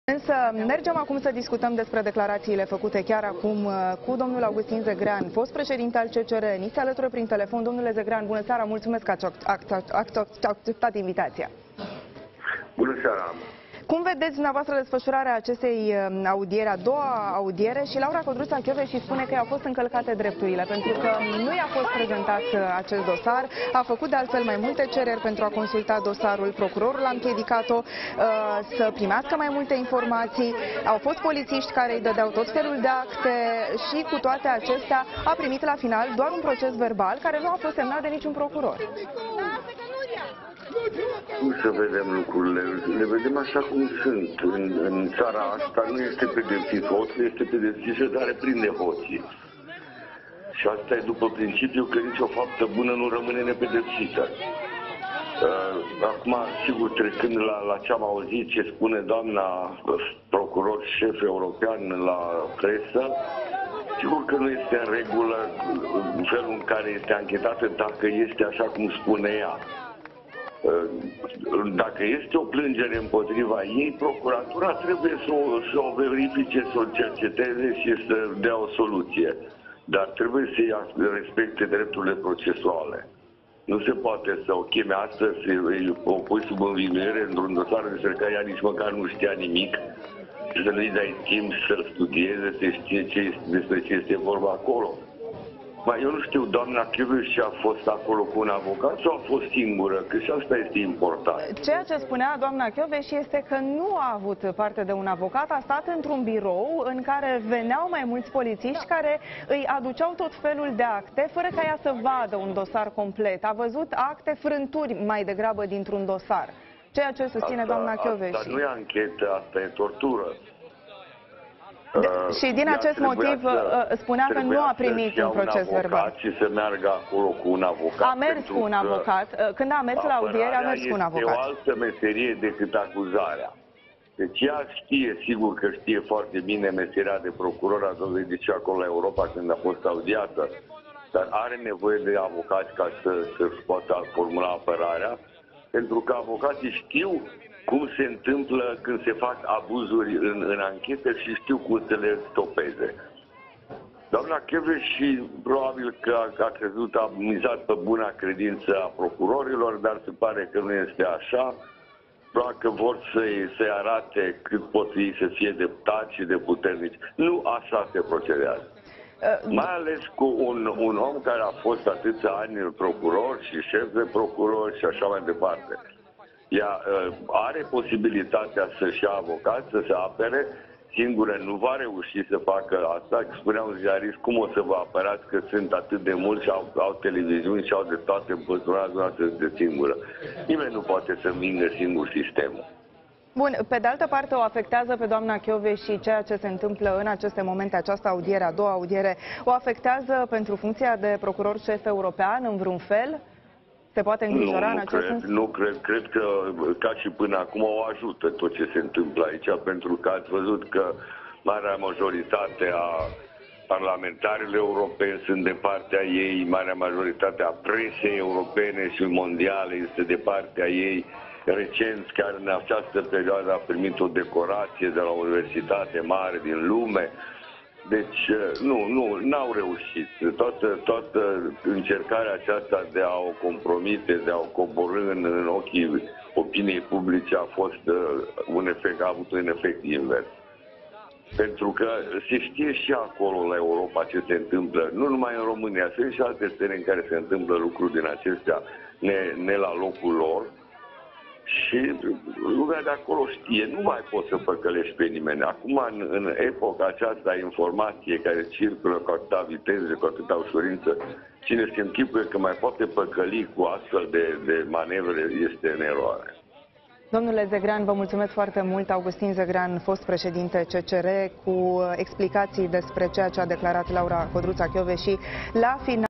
Augustin Zegrean, despre Kovesi la Secția Specială: Asta nu e anchetă, asta e tortură
Augustin Zegrean a vorbit la Digi24 despre prezența Laurei Codruța Kovesi la Secţia specială de anchetare a magistraţilor. Fostul președinte al CCR a comentat declarațiile lui Kovesi la ieșire și spune că modul în care a s-a procedat cu fosta șefă a DNA nu este în regulă. „Asta nu este anchetă, este tortură”, a declarat Augustin Zegrean.

Augustin Zegrean, fost președinte al Curții Constituționale: „În țara asta nu este pedepsit hoțul, este pedepsit cel care prinde hoții, după principiul că „nicio faptă bună nu rămâne nepedepsită”. Trecând la ce am auzit că spune doamna procuror-șef european, sigur că nu este în regulă. Dacă este o plângere împotriva ei, procuratura trebuie să o verifice, să o cerceteze și să dea o soluție. Dar trebuie să îi respecte drepturile procesuale.

Nu se poate să o chemi astăzi, să o pui sub învinuire într-un dosar despre care nici măcar nu știa nimic, să nu-i dai timp să-l studieze, să afle despre ce este vorba.
Dacă este așa cum a povestit, (i se aducea câte un act incomplet din când în când, într-un birou – n.red.), asta nu-i anchetă, asta e tortură. Trebuie să meargă acolo cu un avocat. Avocații știu când se fac abuzuri în anchete și știu cum să le stopeze.
Doamna Kovesi cred că a mizat pe buna credință a procurorilor, dar se pare că nu este așa. Dacă vor să-i arate cât pot să fie ei de tari și de puternici... nu așa se procedează. Mai ales cu un om care a fost atâția ani procuror și șef de procurori. Ea are posibilitatea să se apare cu un avocat, singură nu va reuși. Nimeni nu poate să învingă singur sistemul.
Cât despre funcția de procuror-șef european, cred că această anchetă de fapt o ajută. Ați văzut că cei mai mulți dintre parlamentarii europeni sunt de partea ei, marea majoritate a presei europene și mondiale la fel, a primit o decorație de la universitate mare în lume (Harvard – n. red.). Toată încercarea de a o compromite a avut un efect invers. Se știe în Europa ce se întâmplă aici. Nu mai poți să păcălești pe nimeni.”

Fosta șefă a DNA spune că va dovedi că este nevinovată și a reiterat că aceste șicane din partea secției speciale pentru magistrați au legătură cu candidatura sa la șefia Parchetului European.
Ea spune că i s-au încălcat drepturile și nu s-au respectat procedurile. Laura Codruța Kovesi a precizat, la ieşirea de la audieri, că în primul dosar a cerut să aibă acces la documente, iar procurorul i-a fixat perioada 8-14 martie, astfel că astăzi nu putea să facă nicio declaraţie.